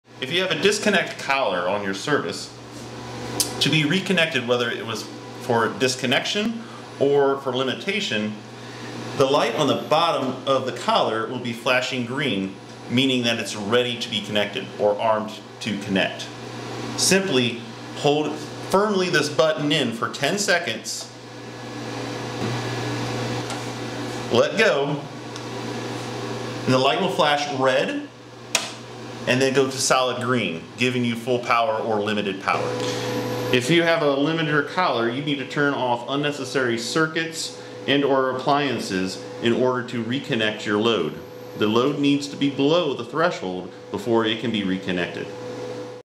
Resetting your Meter after Disconnection or Tripped Limiter -- Meter Collar.
Use the following instructions if this should happen in order to restore power:
- Turn everything off in your home except for a few lights.
- Go out to your meter.
- Reset the device using the reset button, as indicated in the video (This video is for meters equipped with a button on the bottom of your meter.)

When the limiter is off, the light will be red. When the reset is held, the light will be flashing green. When it has been reset, it will be solid green.
You may have to hold the button for a few moments. Give the meter a few minutes to reset.

Go in and turn on a limited number of electric items.